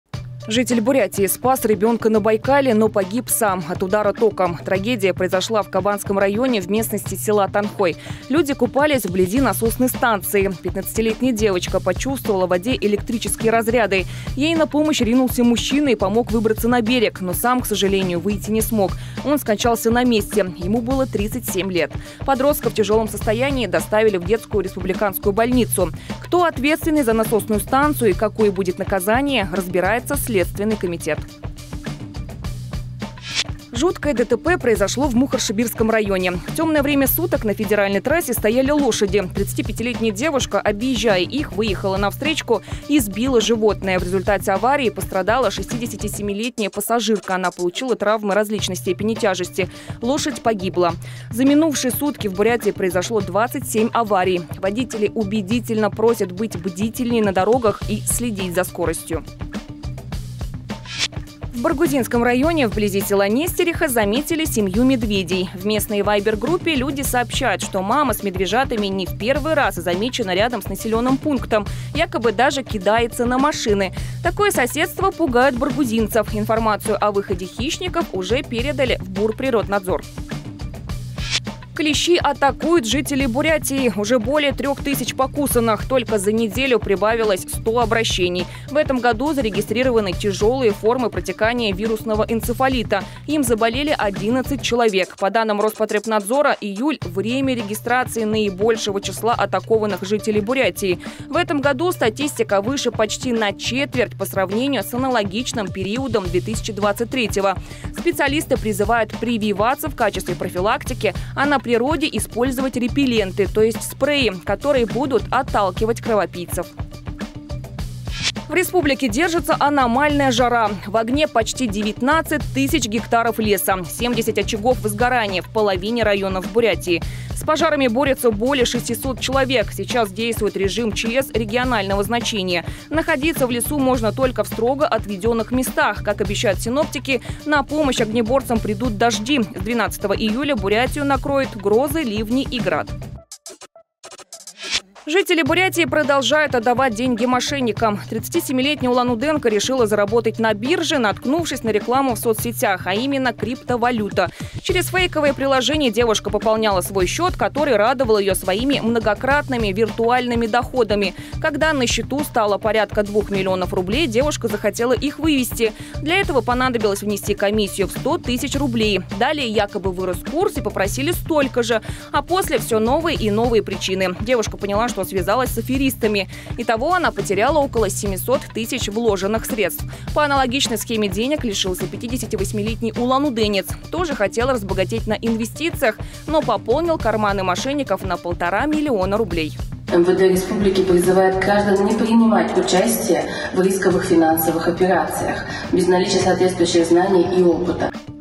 Главное за день: смерть на Байкале, жуткое ДТП и медведи в баргузинской деревне
- Житель Бурятии спас ребёнка на Байкале, но погиб сам от удара током.
- Жуткое ДТП произошло в Мухоршибирском районе.
- В Баргузинском районе вблизи села Нестериха заметили семью медведей.
- Клещи атакуют жителей Бурятии – уже более 3 тысяч покусанных.
- В республике держится аномальная жара. В огне почти 19 тысяч гектаров леса.
- Жители Бурятии продолжают отдавать деньги мошенникам.